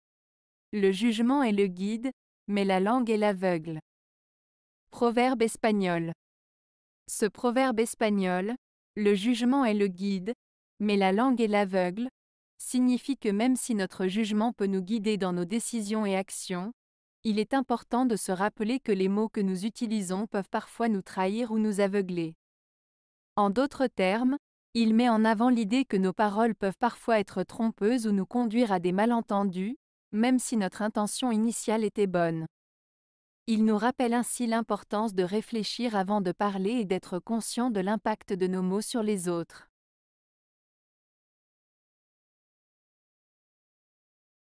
Proverbe Espagnol - Le jugement est le guide, mais la langue est l'aveugle.
Ce proverbe espagnol "Le jugement est le guide, mais la langue est l'aveugle" signifie que même si notre jugement peut nous guider dans nos décisions et actions, il est important de se rappeler que les mots que nous utilisons peuvent parfois nous trahir ou nous aveugler. En d'autres termes, il met en avant l'idée que nos paroles peuvent parfois être trompeuses ou nous conduire à des malentendus, même si notre intention initiale était bonne. Il nous rappelle ainsi l'importance de réfléchir avant de parler et d'être conscient de l'impact de nos mots sur les autres.